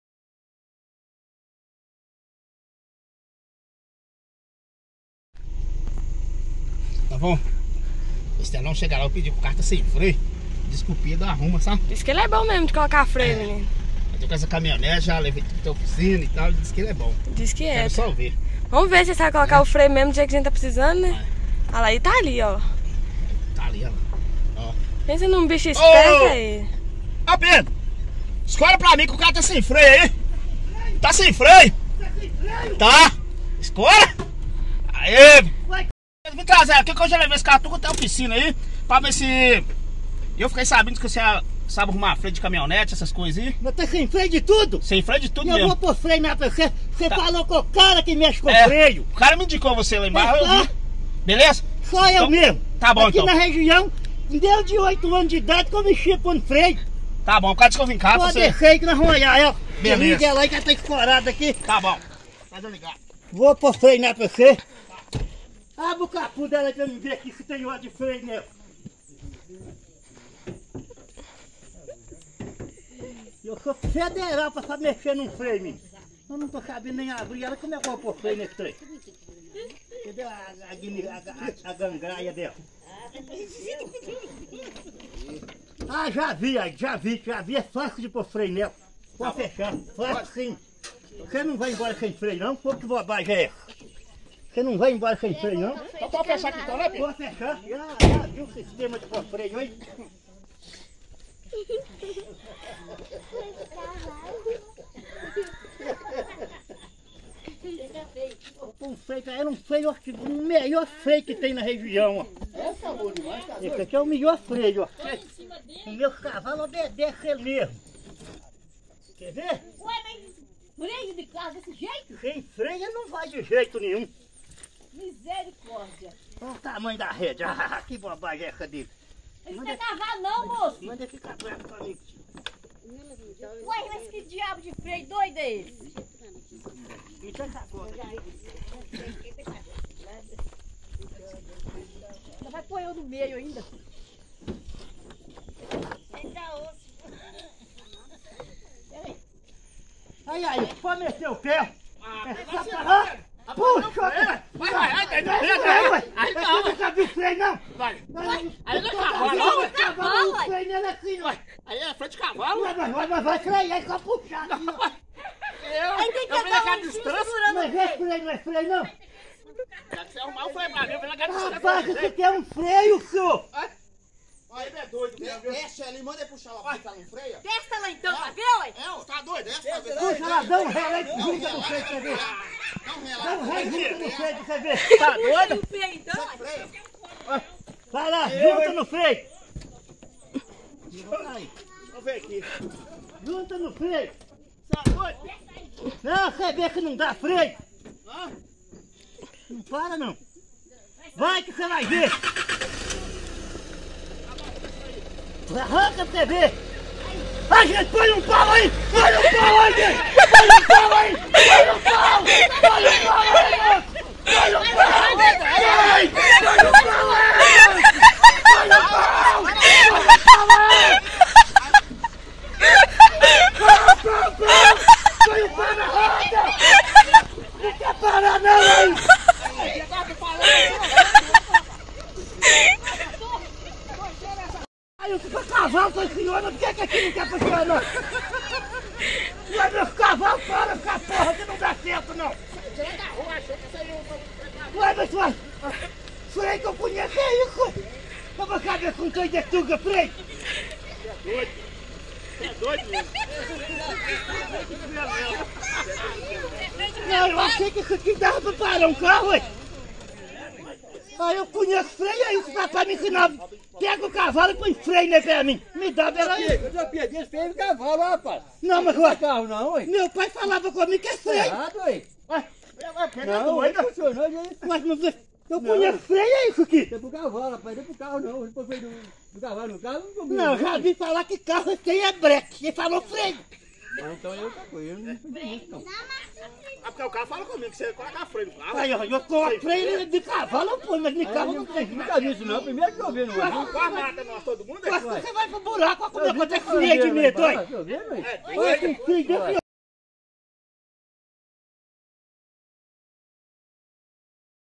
Seu Pedro Trocando Freio da Caminhonete | Batendo Feijão com Fogo 🔥
Vídeo de Pedro Maia | Seja Membro do Canal e fique por dentro de Conteúdos Exclusivos

Agência 20304013
CEP 99999 FZ
(Guanhães MG)